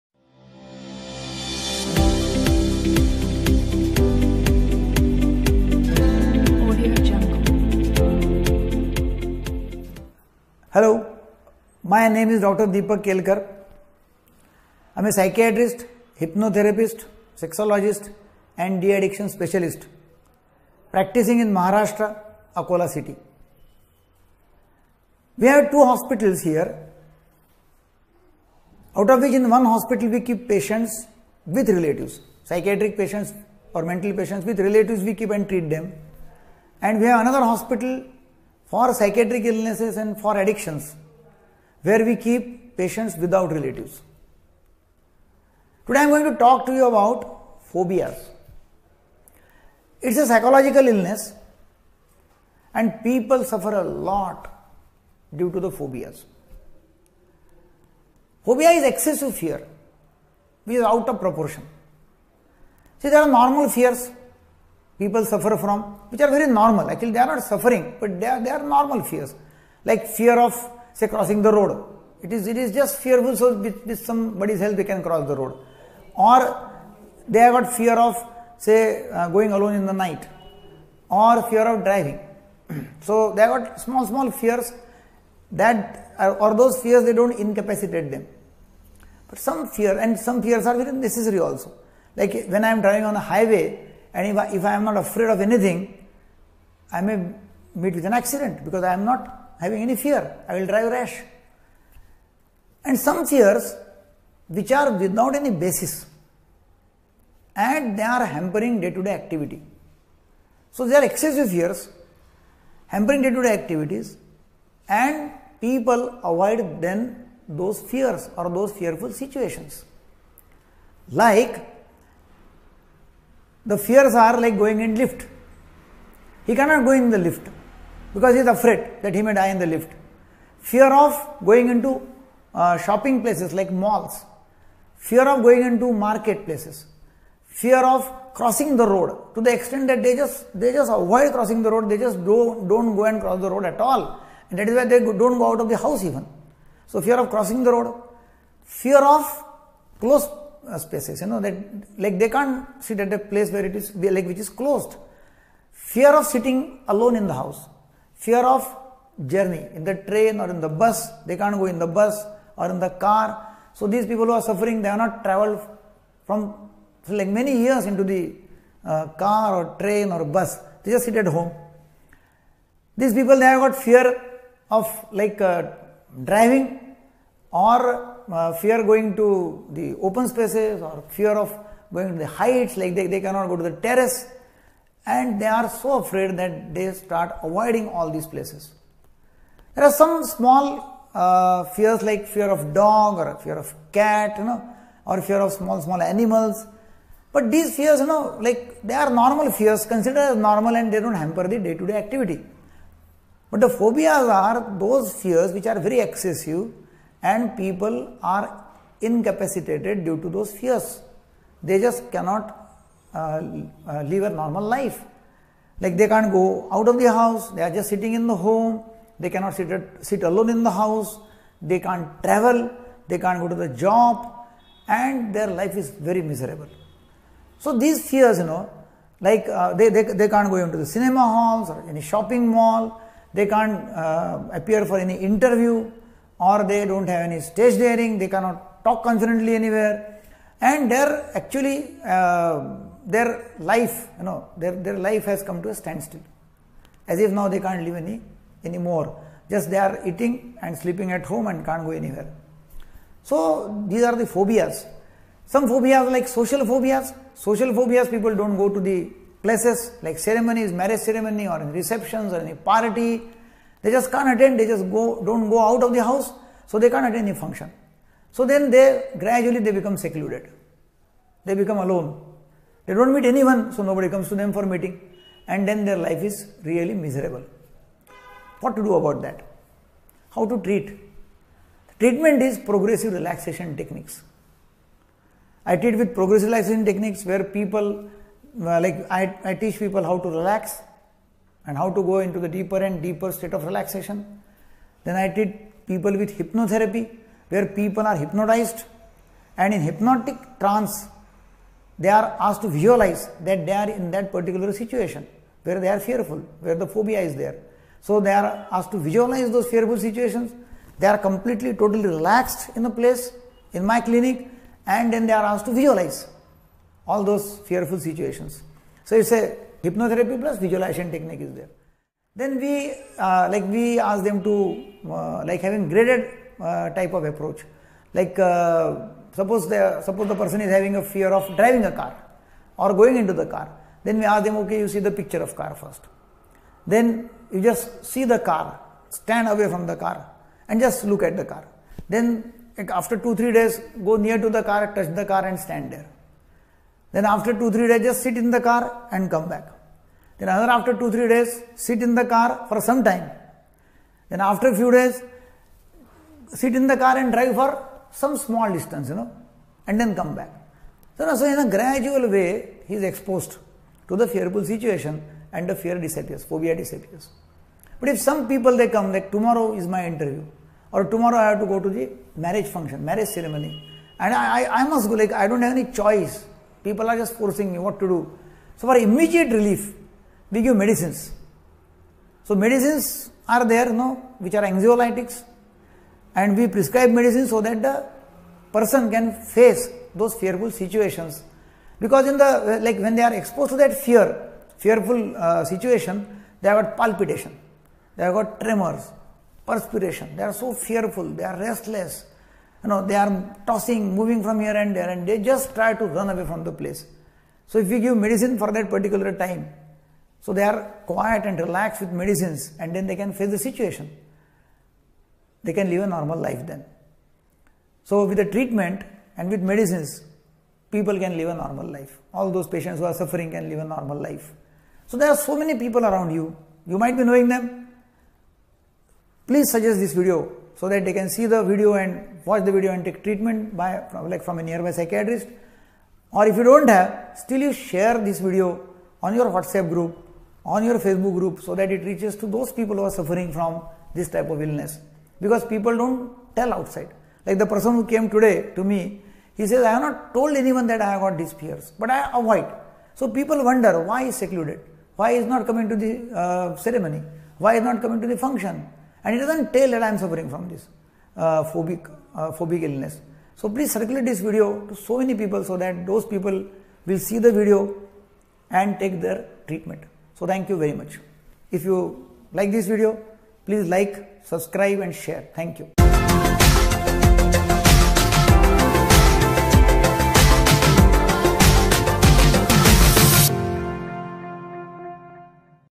Phobia Signs and Symptoms English Motivational Video by Dr Deepak Kelkar
• Psychiatrist
• Hypnotherapist
• Psychotherapist,
• De-addiction Specialist
• Anxiety and Depression ( Counselling )